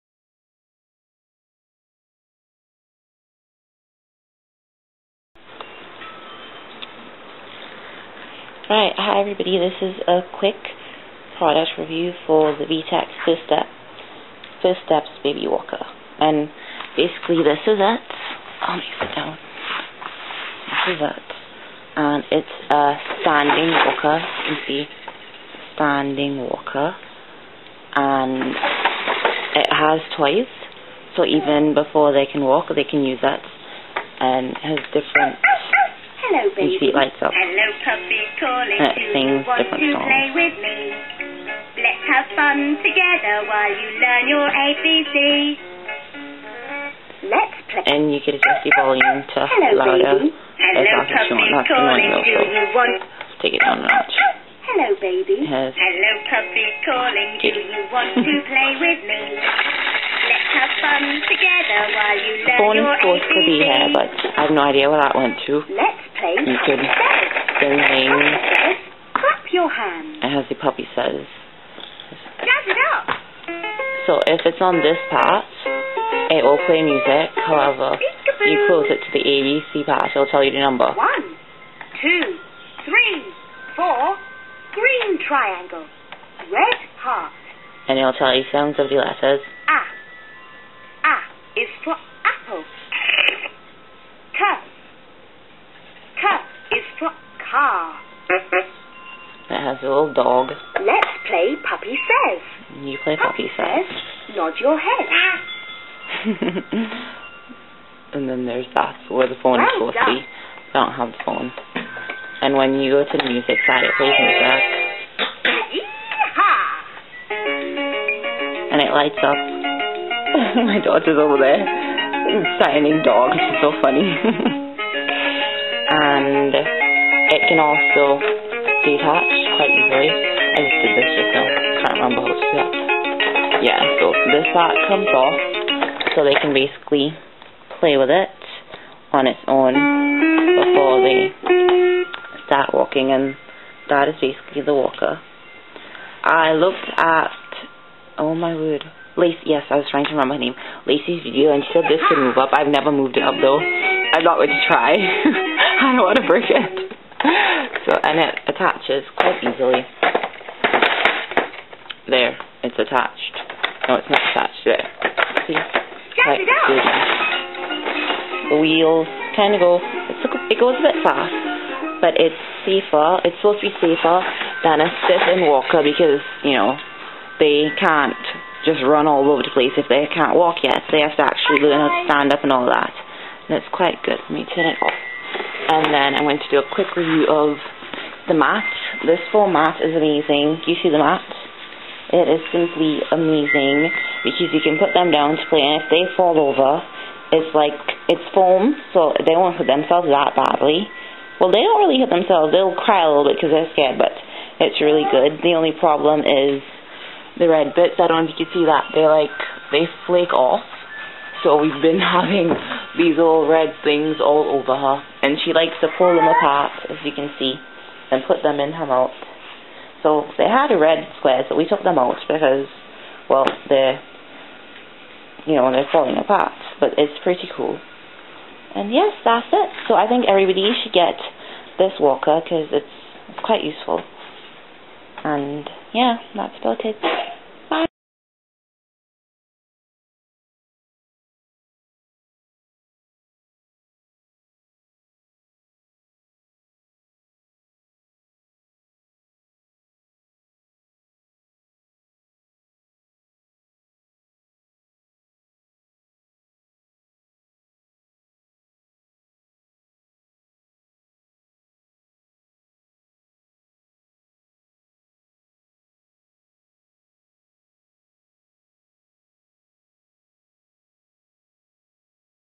Vtech first steps baby walker and Alphabet & Number Puzzle mat product review
vtech first steps baby walker product review

Alphabet and Number Puzzle Mat